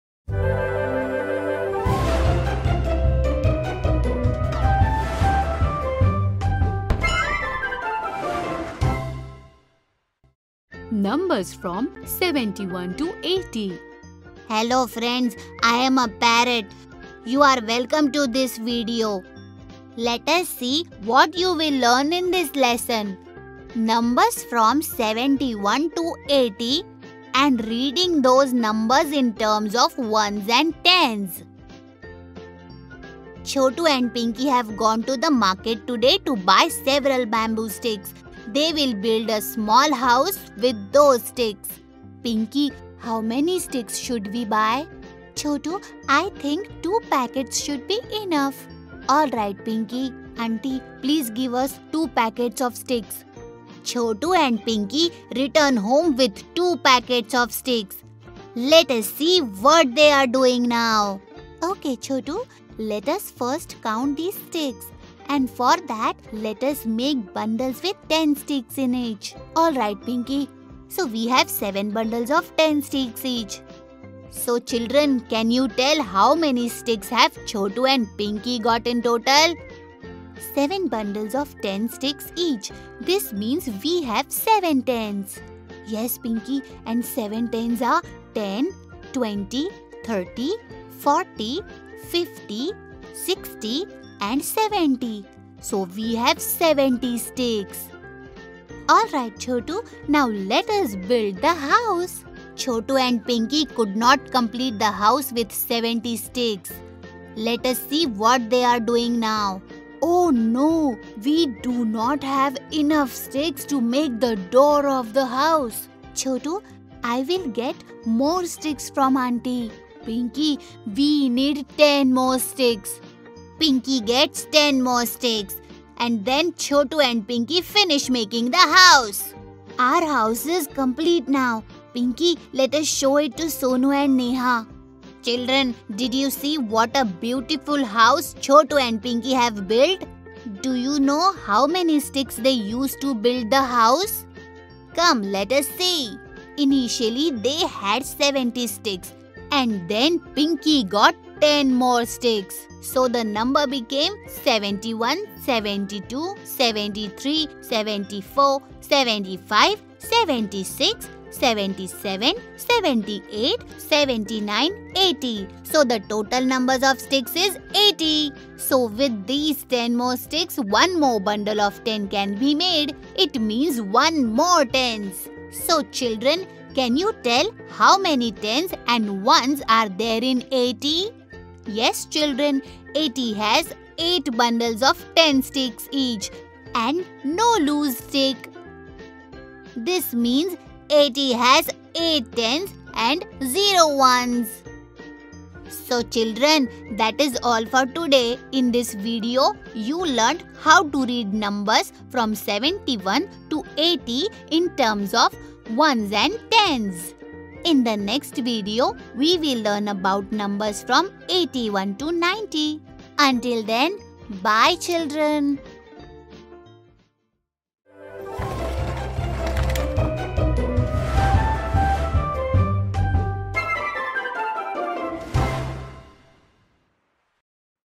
Numbers from 71-90 | Part 1/3 | English | Class 1
Numbers from 71-90 | Part 1/3  | English | Class 1 |  | NCERT |

Numbers from 71-90 | Two digit numbers | Grade 1 | NCERT | Numbers

Welcome to part 1 of Numbers from 71-90. In this video, we will discuss the concept of Numbers from 71-90.